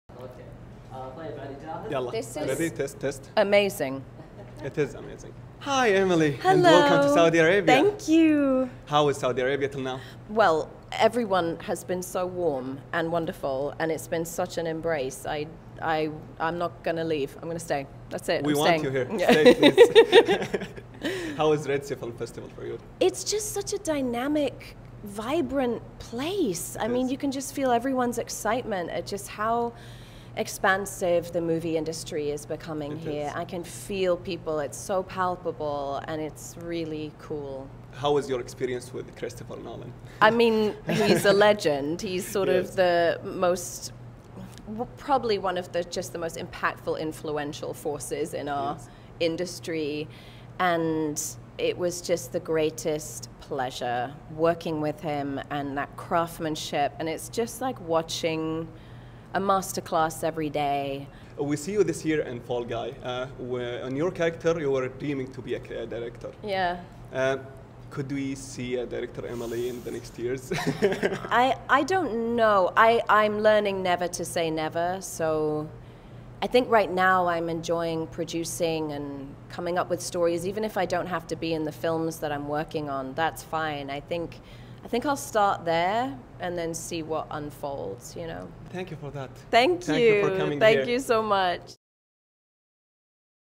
هل من الممكن أن نرى النجمة إيميلي بلنت بدور المخرج؟ 🎬🤔
في مقابلة حصرية من قلب حفل مهرجان البحر الأحمر السينمائي الدولي، تكشف النجمة العالمية إيميلي بلنت عن رؤيتها المستقبلية واحتمالية تحولها إلى دور المخرج. اكتشفوا معنا التفاصيل الشيقة وأسرار مسيرتها الفنية!

لمعرفة المزيد من أخبار الترفيه أولاً بأول، يمكنك متابعة حسابتنا على مواقع التواصل الاجتماعي: